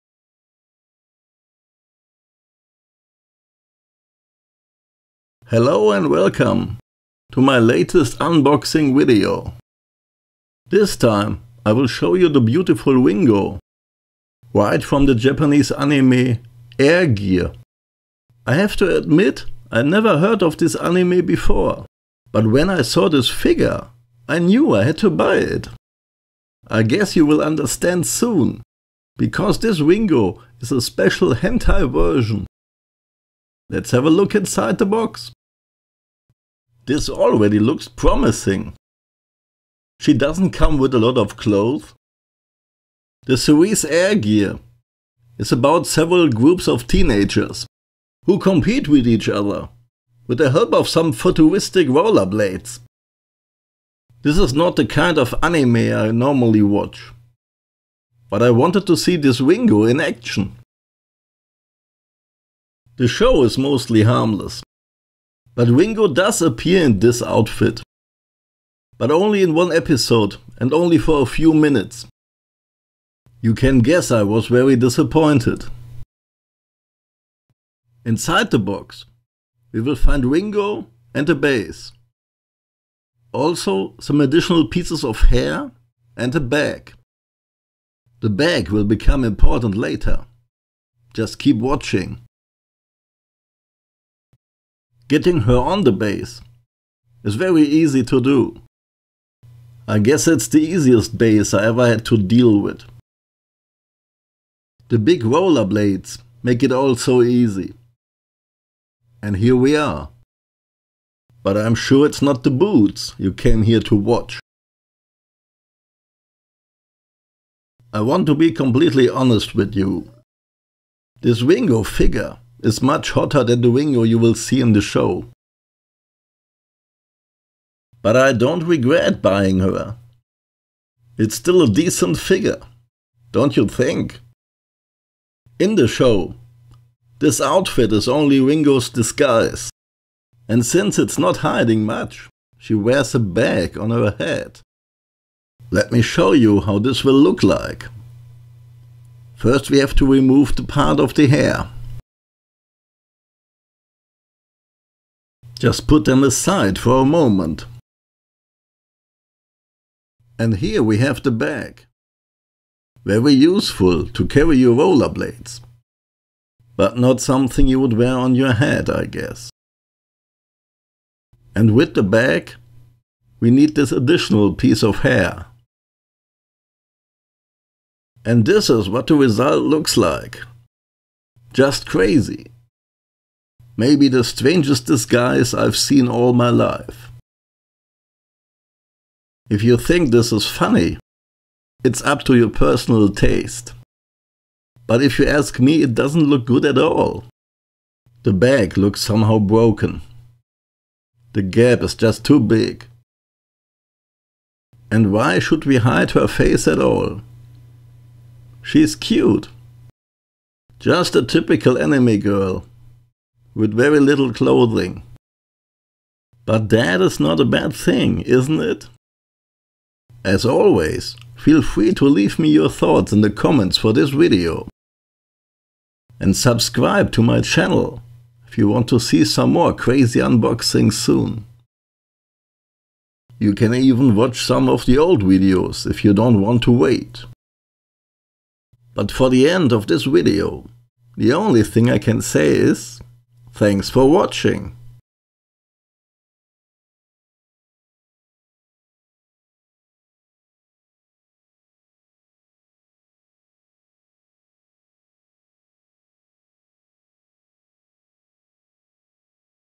Hed's unboxing and review of Air Gear - Noyamano Ringo - Hentai Version by Q-SIX
Hello loyal viewers!
I am glad you are back for a new video of Hed's Unboxing!

In this video I will show you a figure of Noyamano Ringo from the anime »Air Gear«. As you can see Ringo doesn’t wear much clothing  because it is the special hentai version. The figure was made by the Japanese company Q-SIX.